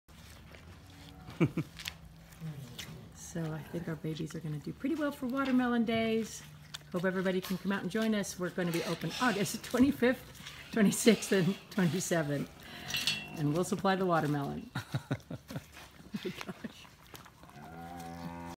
Camel vs Watermelon
Oasis Camel Dairy Watermelon Days Aug. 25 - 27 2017